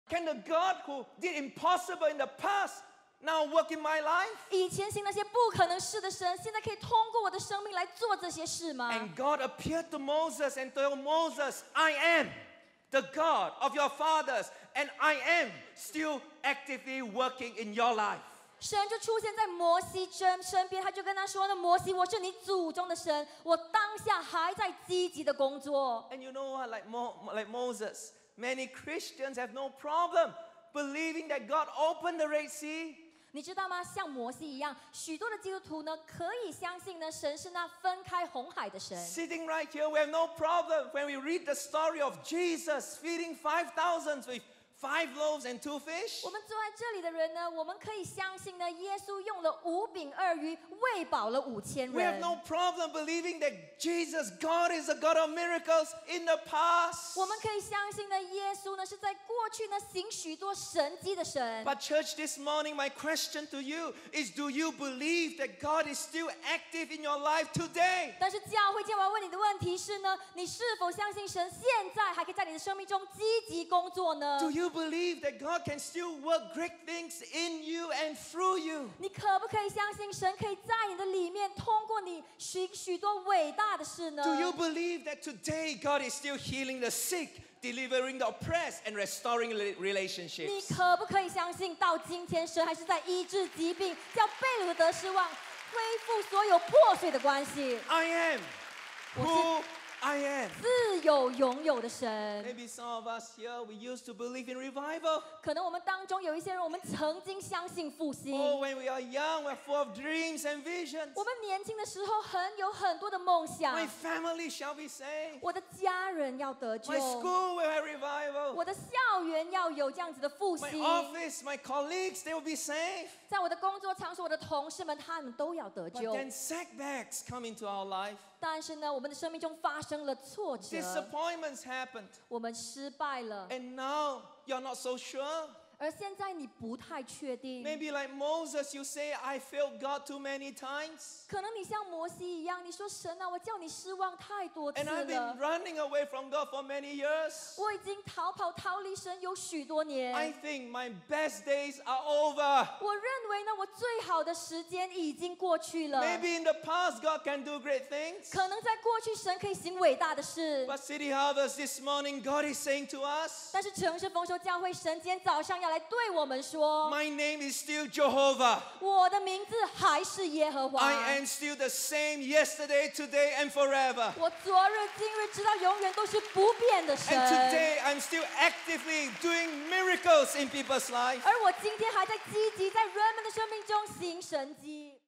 Bobby Chaw: Jehovah / 耶和华 (Bilingual)
Highlights of sermon preached by Pastor Bobby Chaw at City Harvest Church.
Watch or listen to the full sermon at The CHC App

Following his message on the name "El Shaddai", Bobby Chaw teaches on another name of God in this sermon: "Jehovah". A name created out of the most holy YHWH, Jehovah—"I AM WHO I AM" is the Lord God who sent Moses to set the children of Israel free. This sermon teaches you four things about "Jehovah": He is active in the present, He is with us to the end of the age; He is the God of revelation, He desires to reveal Himself; He is holy and righteous, so believers must be holy; but He is also merciful and gracious. This message will refresh your understanding of the Jehovah God who loves you and wants you to draw near with a true heart.

继上一篇信息之后，赵仁牧师再教导神的另外一个名字——“耶和华”。这个名字是从神最圣洁的名——YHWH——而来。耶和华——“我是自有永有的”——就是那位差派摩西叫以色列百姓得自由的神。这篇道教导我们关于耶和华的四件事：祂当下也在积极工作、祂与我们同在，直到世界的末了、祂是启示的神，渴望显现自己、祂是圣洁公义的，因此信徒必须圣洁，但祂也是充满怜悯与恩惠的神。这篇信息将更新你对耶和华的理解——祂爱你，并希望你带着真实的心亲近祂。